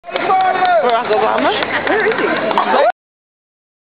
obama leaves gym
obama leaving the gym in albuquerque. don't blink or you'll miss him. he waves, i drop the camera. good one.